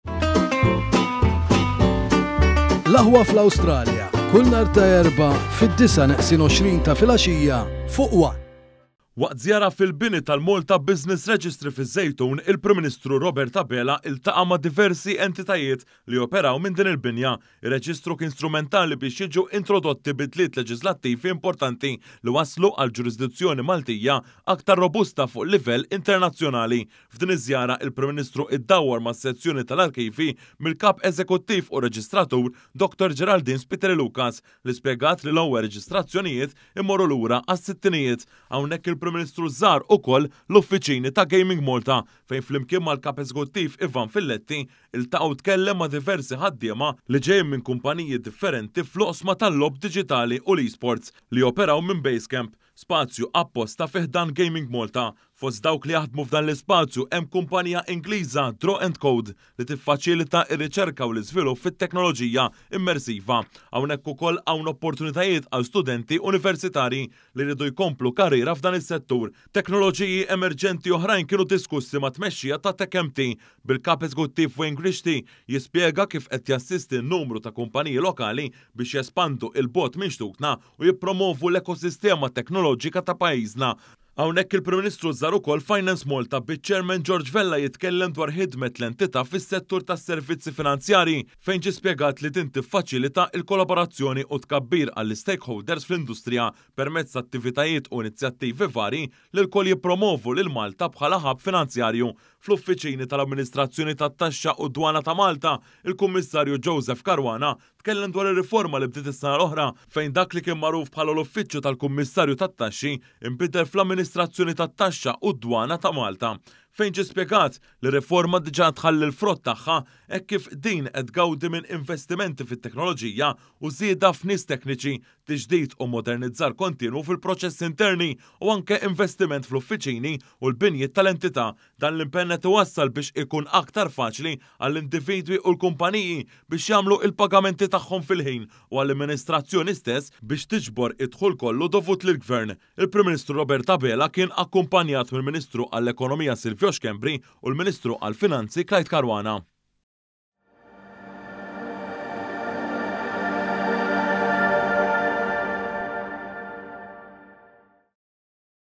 IL-PM IŻUR IL-ĦADDIEMA FIL-BINI TAL-MBR
ONE.COM.MT •
Waqt żjara fil-bini tal-Malta Business Registry fiż-Żejtun, il-Prim Ministru Robert Abela ltaqa’ ma’ diversi entitajiet li joperaw minn din il-binja.

Ir-Reġistru kien strumentali biex jiġu introdotti bidliet leġiżlattivi importanti, li wasslu għal ġurisdizzjoni Maltija aktar robusta fuq livell internazzjonali.

F’din iż-żjara, il-Prim Ministru ddawwar mas-sezzjoni tal-arkivji mill-Kap Eżekuttiv u r-Reġistratur Dr Geraldine Spiteri Lucas, li spjegat li l-ewwel reġistrazzjonijiet imorru lura għas-sittinijiet.

Hawnhekk il-Prim Ministru żar ukoll l-uffiċini ta’ Gaming Malta, fejn, flimkien mal-Kap Eżekuttiv Ivan Filletti, iltaqa’ u tkellem ma’ diversi ħaddiema ġejjin minn kumpaniji differenti fl-oqsma tal-logħob diġitali u l-esports, li qed joperaw minn Basecamp, spazju apposta fi ħdan Gaming Malta.

Fost dawk li jaħdmu f’dan l-ispazju hemm kumpanija Ingliża Draw&Code, li tiffaċilita r-riċerka u l-iżvilupp fit-teknoloġija immersiva. Hawnhekk ukoll hawn opportunitajiet għal studenti Universitarji li jridu jkomplu karriera f’dan is-settur.

Teknoloġiji emerġenti oħrajn kienu diskussi mat-tmexxija ta’ Tech.mt, bil-Kap Eżekuttiv Wayne Grixti jispjega kif qed tassisti numru ta’ kumpaniji lokali biex jespandu ’l bogħod minn xtutna u jippromwovu l-ekosistema teknoloġika ta’ pajjiżna.

Hawnhekk il-Prim Ministru żar ukoll Finance Malta, biċ-Chairman George Vella jitkellem dwar ħidmet l-entità fis-settur tas-servizzi finanzjarji, fejn ġie spjegat li din tiffaċilita l-kollaborazzjoni u t-tkabbir għall-istakeholders fl-industrija permezz t’attivitajiet u inizjattivi varji li lkoll jippromwovu lil Malta bħala hub finanzjarju.

Fl-uffiċini tal-Amministrazzjoni tat-Taxxa u d-Dwana ta’ Malta, il-Kummissarju Joseph Caruana tkellem dwar ir-riforma, li bdiet is-sena l-oħra, fejn dak li kien magħruf bħala l-Uffiċju tal-Kummissarju tat-Taxxi, inbidel fl-Amministrazzjoni tat-Taxxa u d-Dwana ta’ Malta.

Ġie spjegat li r-riforma diġà qed tħalli l-frott tagħha hekk kif din qed tgawdi minn investimenti fit-teknoloġija u żieda f’nies tekniċi, tiġdid u mmodernizzar kontinwu fil-proċessi interni u anke investiment fl-uffiċini u l-binjiet tal-entità.

Dan l-impenn qed iwassal biex ikun aktar faċli għall-individwi u l-kumpaniji biex jagħmlu l-pagamenti tagħhom fil-ħin u għall-amministrazzjoni stess biex tiġbor id-dħul kollu dovut lill-Gvern.

Il-Prim Ministru Robert Abela kien akkumpanjat mill-Ministru għall-Ekonomija, Silvio Schembri, u mill-Ministru għall-Finanzi, Clyde Caruana.